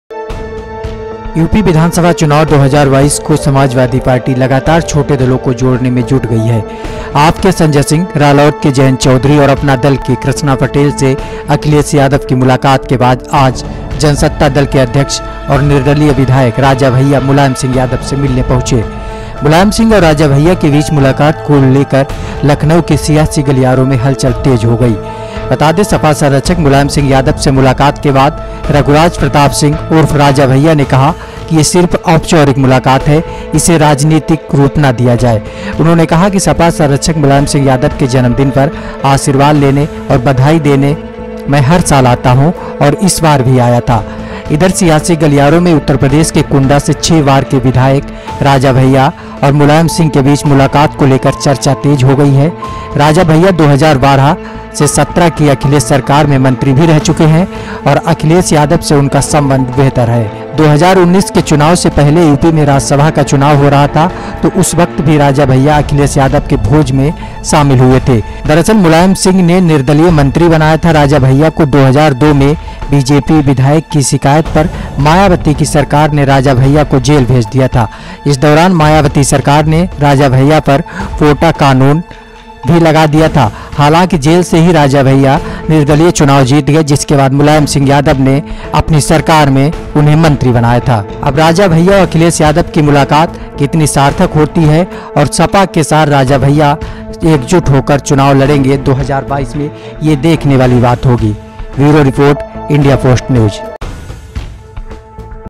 Raja Bhaiya ने की Mulayam Singh से मुलाकात कर बदला राजनीतिक समीकरण ।। Indiapost­­­­ NEWS
Indiapost ­­­­NEWS